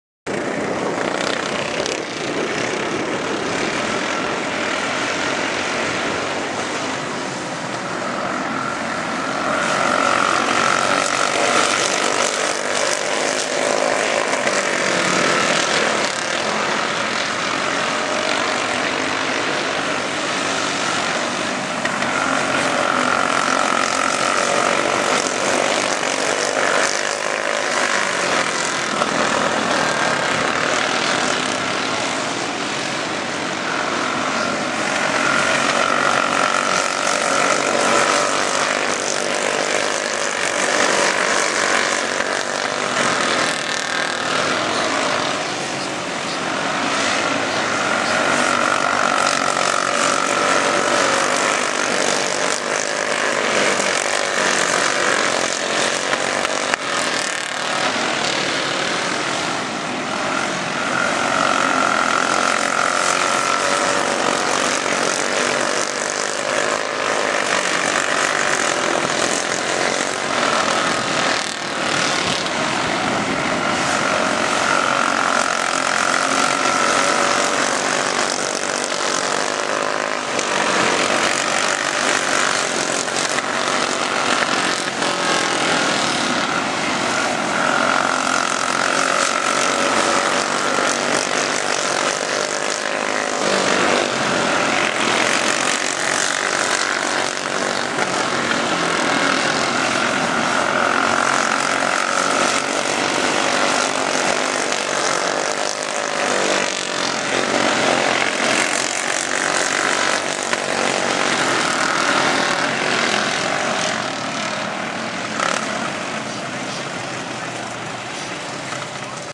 MASTERS MINI DWARFS Heat 1 Barona Speedway 6-22-2024
MASTERS MINI DWARFS
HEAT 1
1 2 2 Brysen Byford Brysen Byford San Diego, CA 1
2 4 08 Shelton Scott Shelton Scott San Diego, CA 2
3 6 7 Brenton Scott Brenton Scott Ramona, CA 3
4 3 8M Grace Escalante Grace Escalante Yuma, AZ -1
5 1 48 Sadielee Talkington Sadielee Talkington Riverside, CA -4
6 5 39M Jackson Reiter Jackson Reiter San Diego, CA -1
All transfer